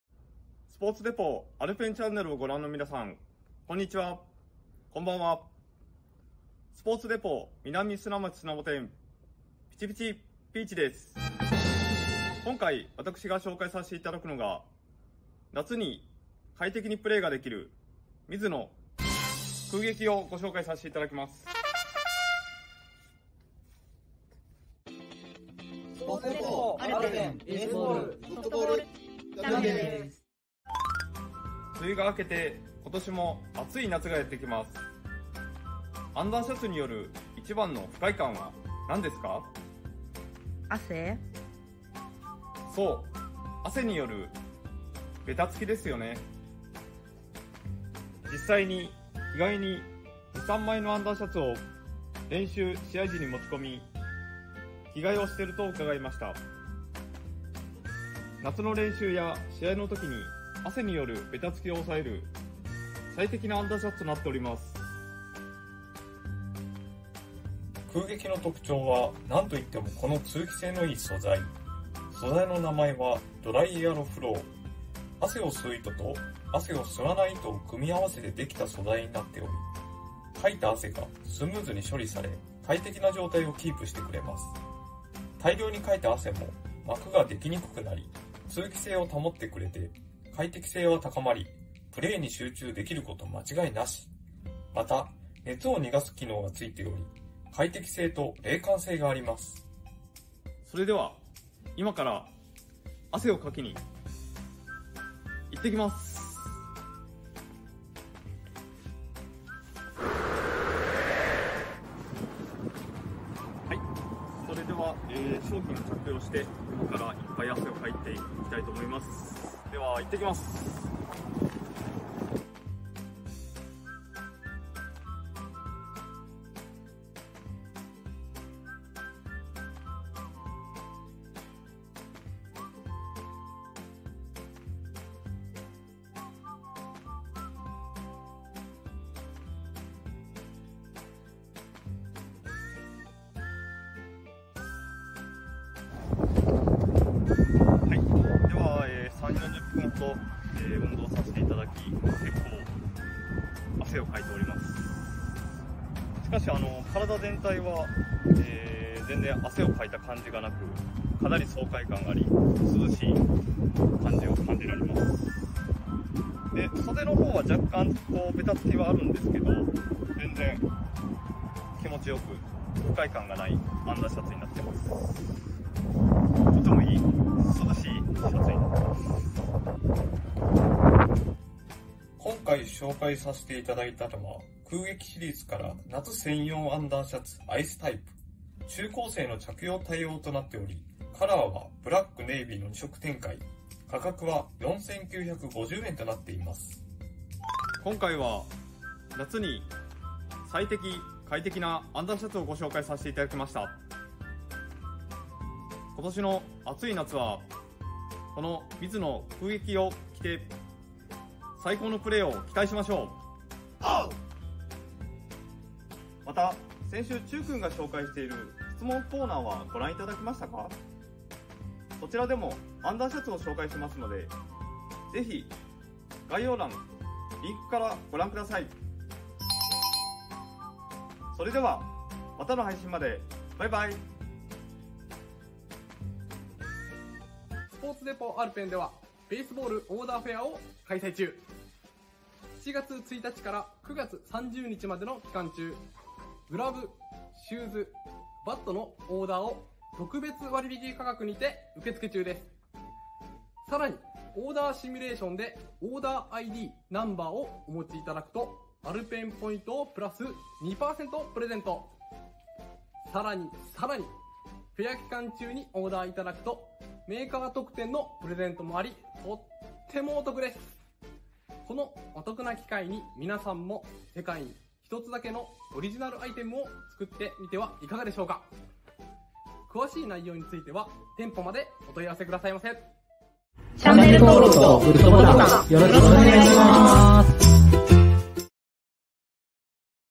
【Baseball・Softball】夏に快適なアンダーウェアの紹介
スポーツデポ・アルペンチャンネル

初登場！南砂町スナモ店のピーチくんです！
今回はアンダーウエアについて説明します
是非、店舗にも遊びに来て下さいね！

【登場商品】
ミズノMZ 12JA2P14KGICh